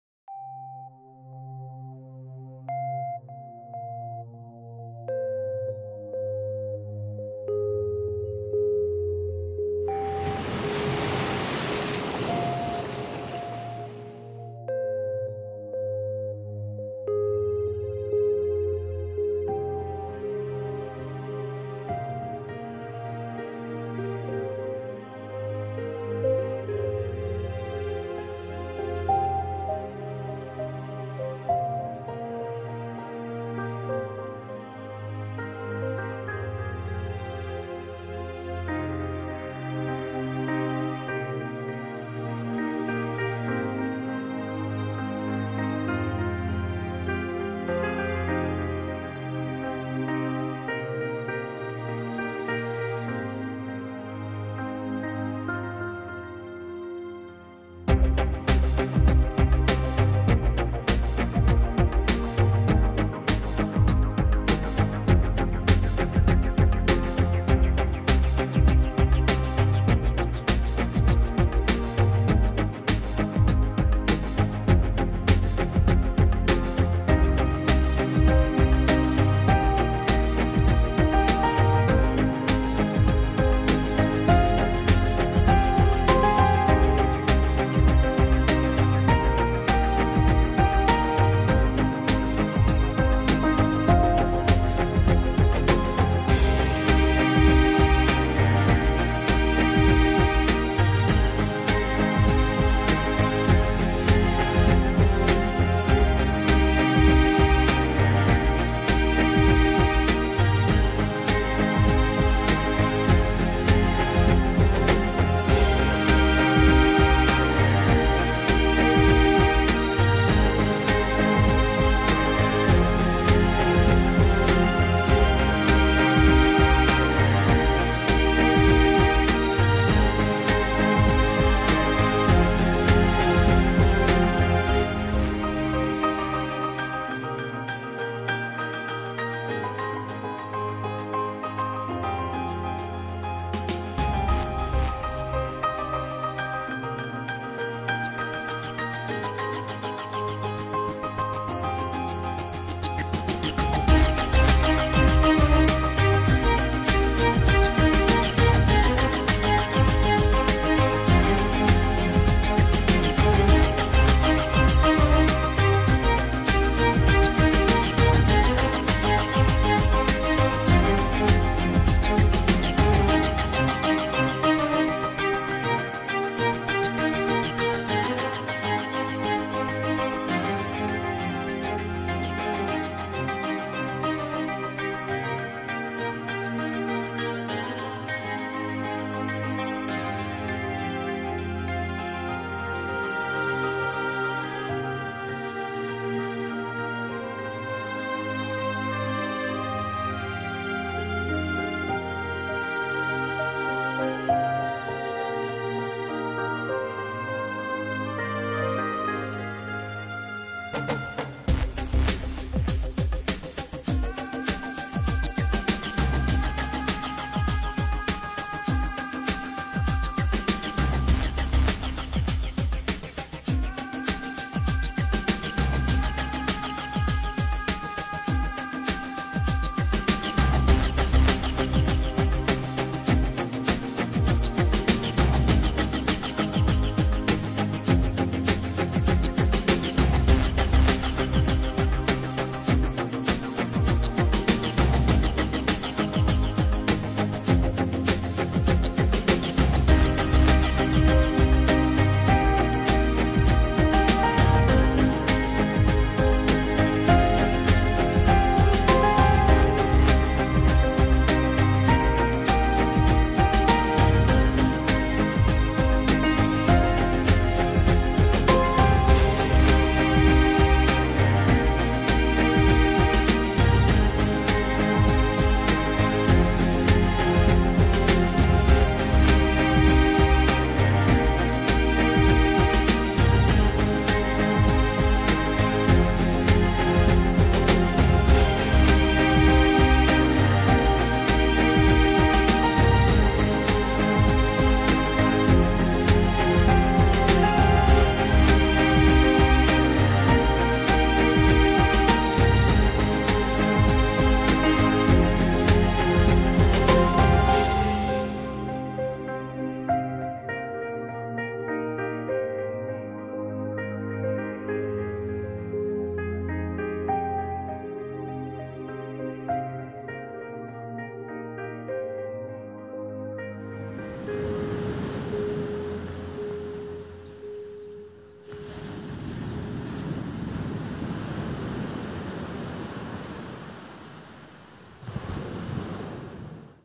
Seychelles by drone - Mahé, La Digue, Praslin
Seychelles are a destination of mindblowing beauty and worlds best beaches. Shots were taken at Praslin, La Digue, Curieuse, Mahe, Mayonne.